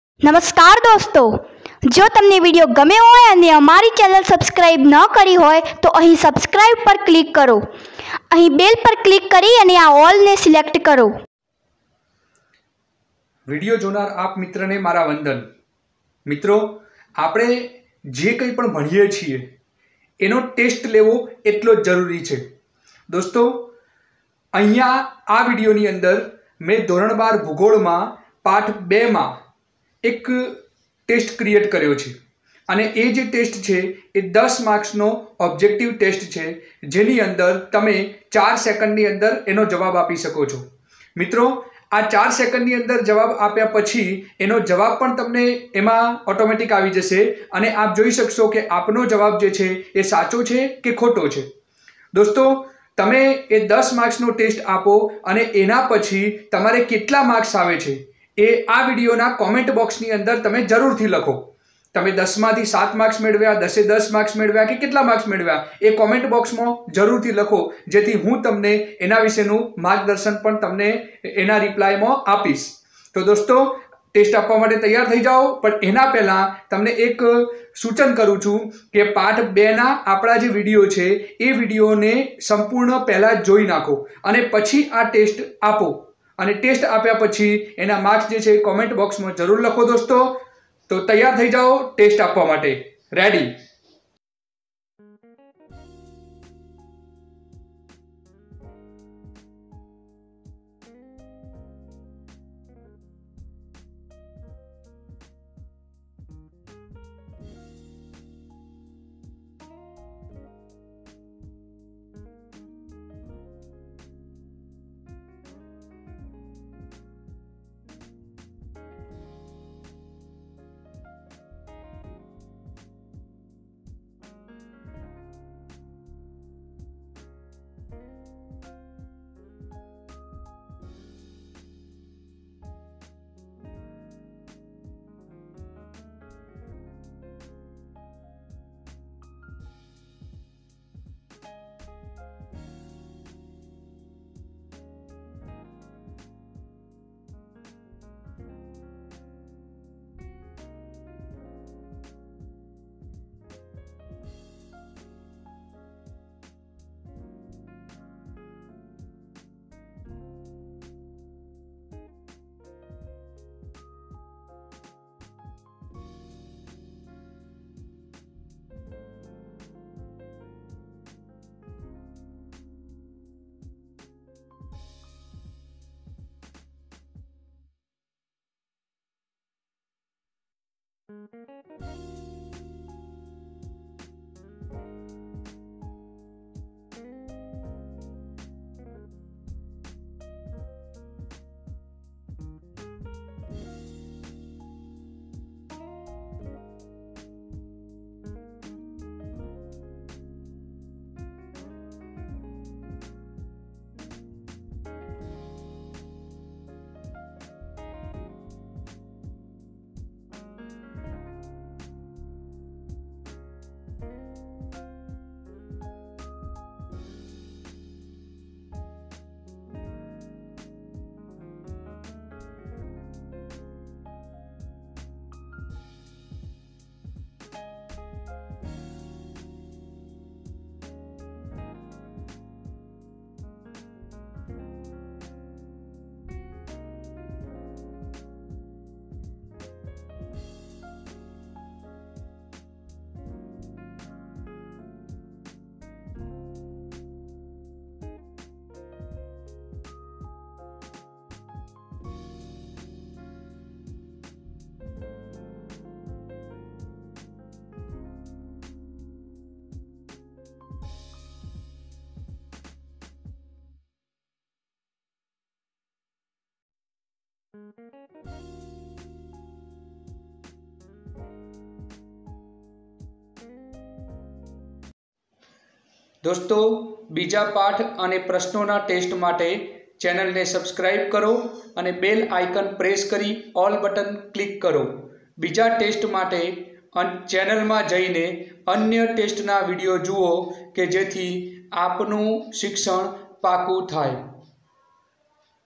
ધોરણ -12 ભૂગોળ|Std-12 Geography,પાઠ -2 માનવ વસ્તી,Online Test, વિભાગ A
Std-12 Geography, chp-2 Manav vasti, IMP QUESTIONS, IMP prashno, one 1 marks questions, 1 marksna tunka prashno, Vibhag A, Taught by : Jagdish Patel